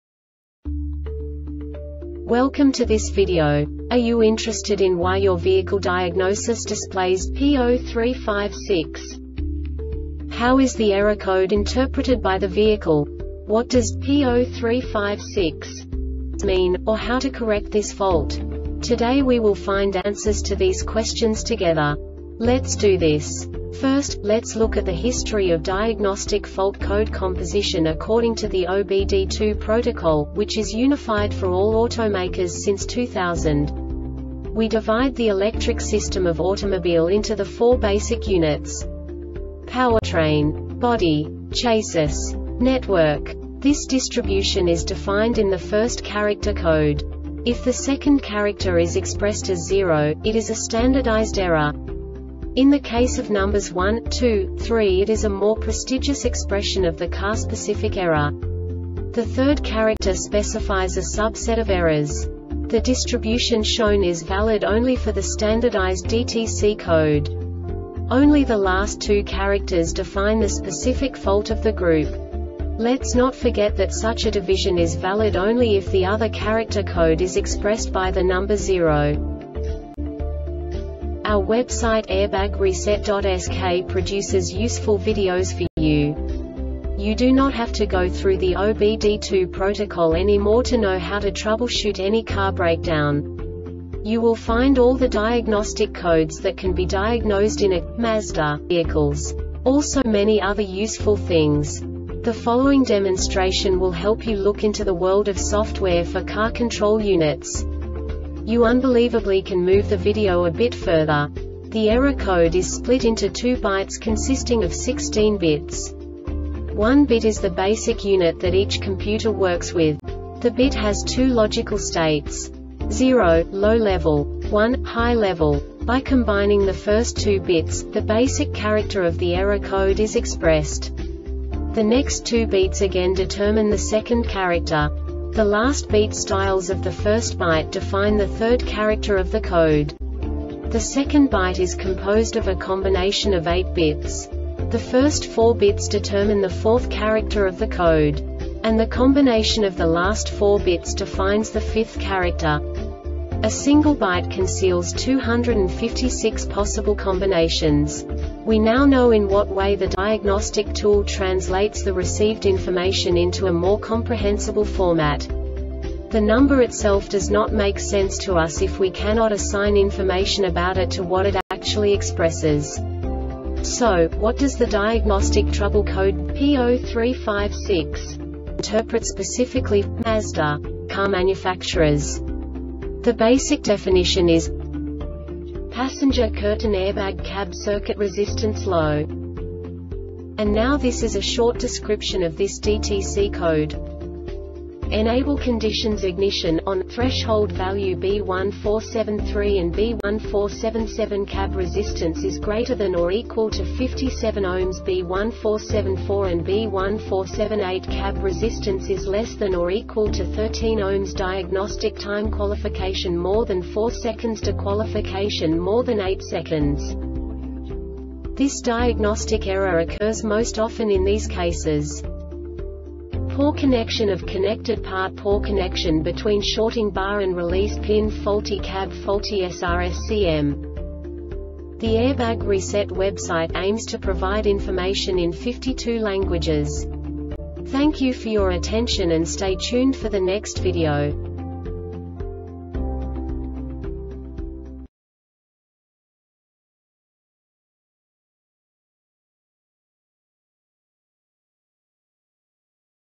DTC Mazda P0356 Short Explanation
Contents:
0:21 Basic DTC analysis according to OBD2 protocol standard.
1:48 Insight into programming
2:58 A diagnostically understandable description of the Diagnostic Trouble Code
3:05 Basic error definition

Make:
Mazda
Code:
P0356
Definition:
Passenger Curtain Airbag (CAB) Circuit Resistance Low
Description:
Enable Conditions Ignition "ON" Threshold Value B1473 and B1477 CAB resistance ≥ 5.7Ω B1474 and B1478 CAB resistance ≤ 1.3Ω Diagnostic Time Qualification More than 4 sec De-Qualification More than 8 sec
Cause:
 Poor connection of connected part. Poor connection between shorting bar and release pin. Faulty CAB. Faulty SRSCM.